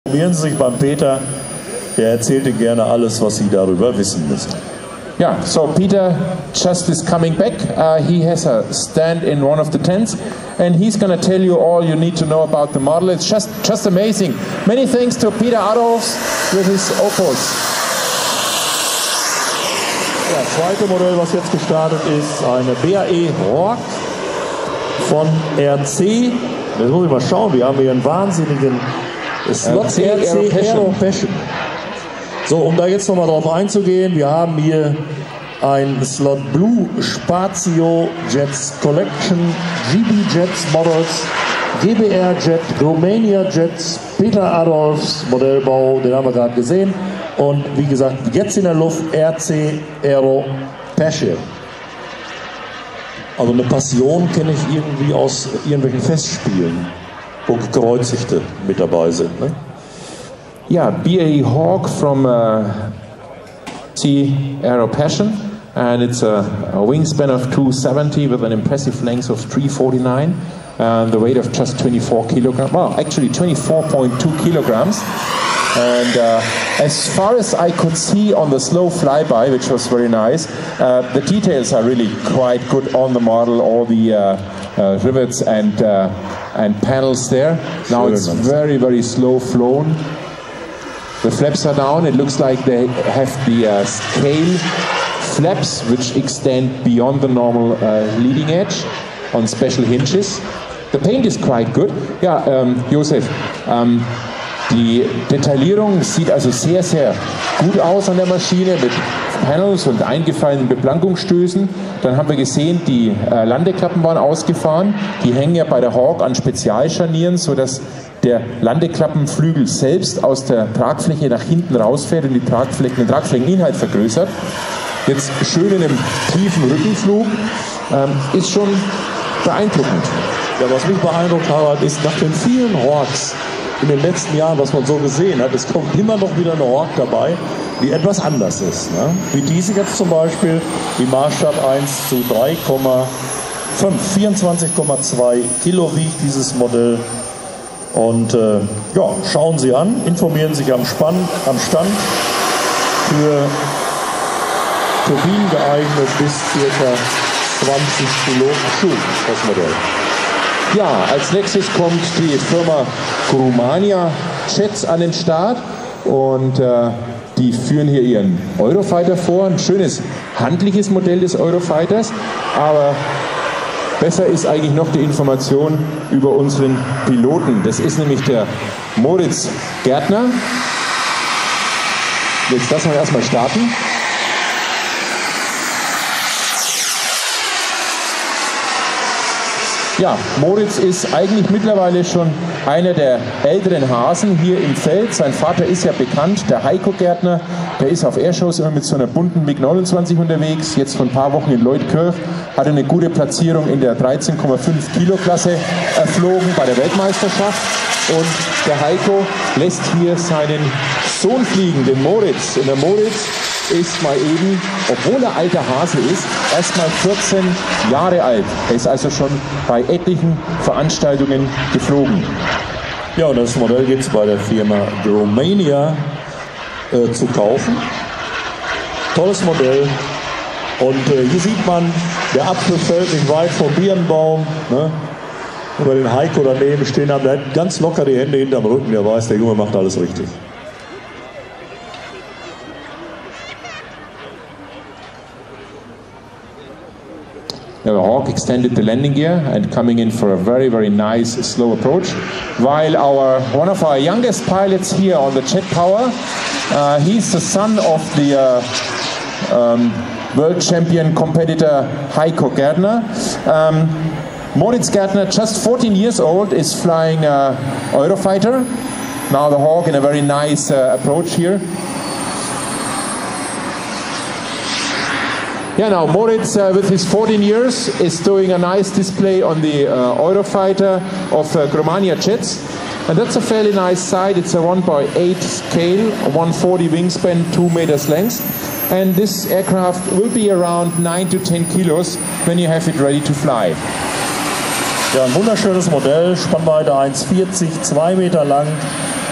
BAE HAWK MK66 HUGE RC SCALE MODEL TURBINE JET FULL DISPLAY DEMO FLIGHT / Jetpower Messe 2015
Wingspan: 1,60m
Length: 3,49m
Scale: 1:3,5
Weight: 24,2 Kg
Engine: ???
Pilot: ???
Event: RC Airshow / Jetpower Fair in Bad Neuenahr Ahrweiler Germany September 2015